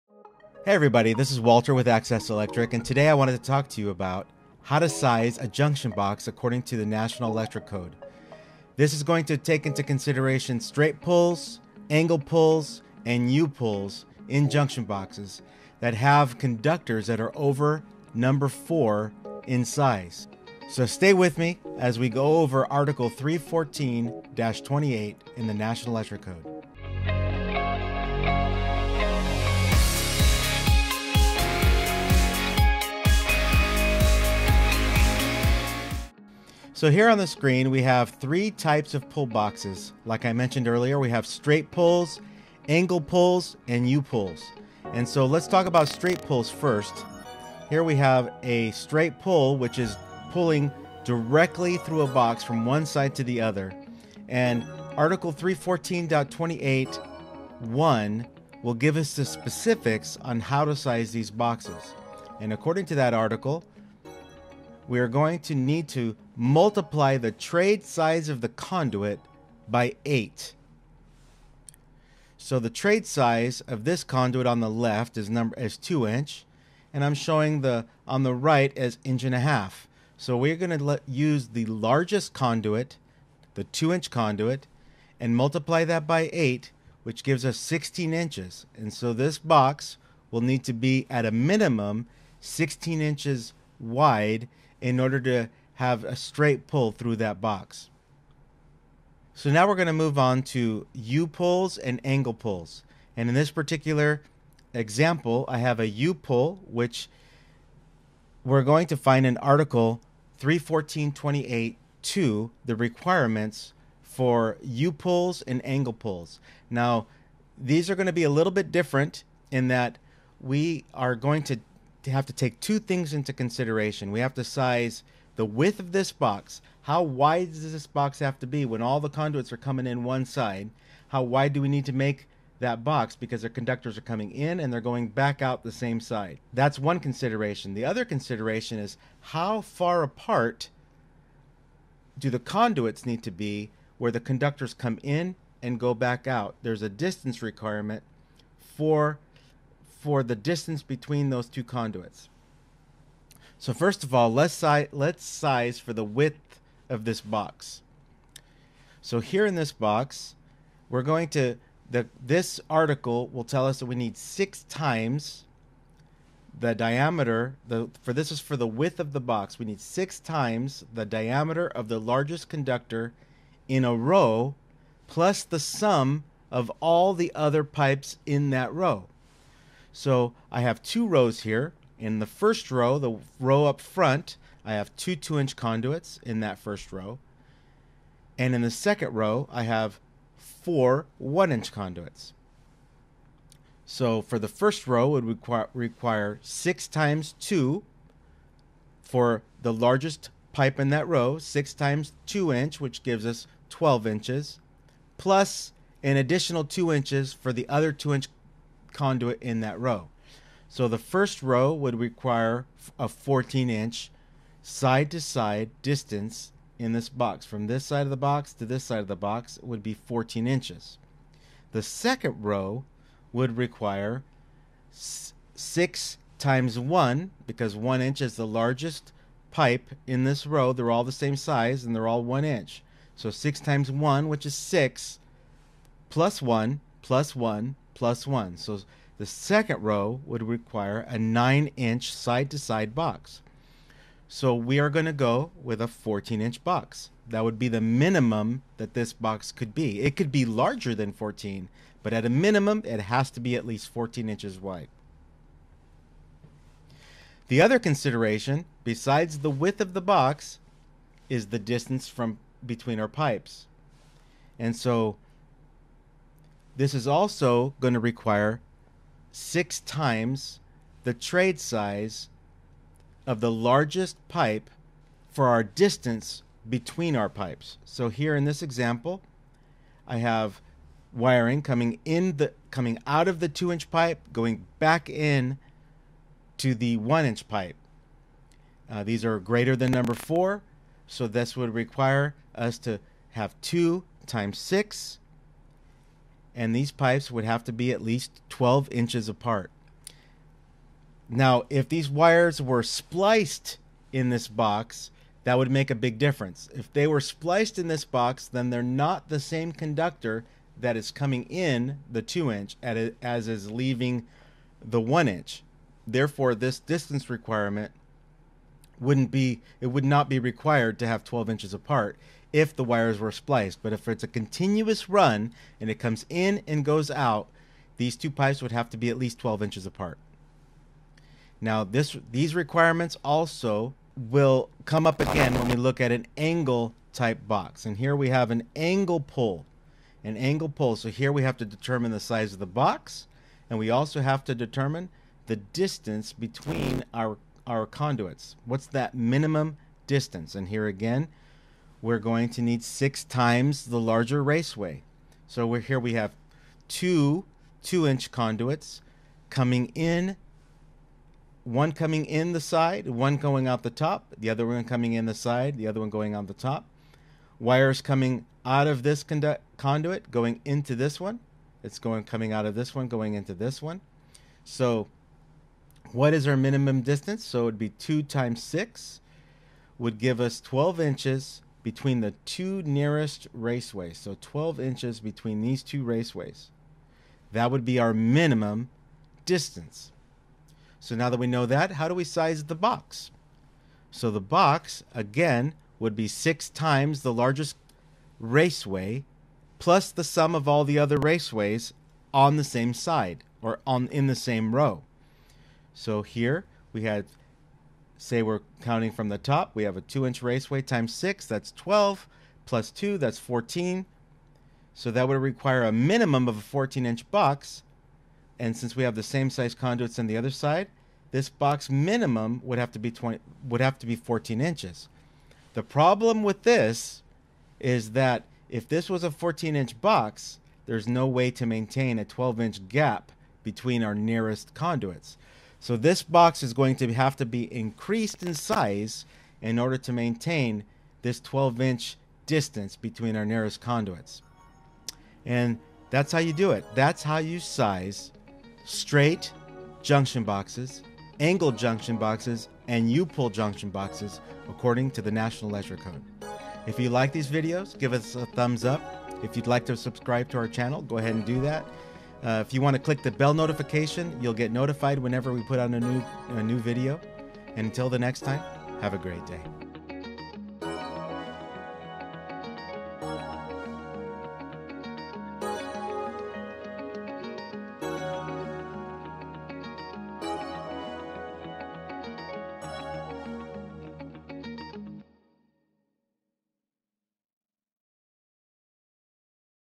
Sizing Pull and Junction Boxes made Easy
In this episode of AccessToPower we discuss the basics of how to size pull boxes according to the NEC, or National Electrical Code, correctly. The exact location dictating how to size junction boxes in the Code book can be found in Article 314.28. All the requirements discussed in this video are found in that code section. The object of this episode is to help electricians and apprentices to learn the basics of pull box sizing for enclosures holding #4 AWG conductors and greater.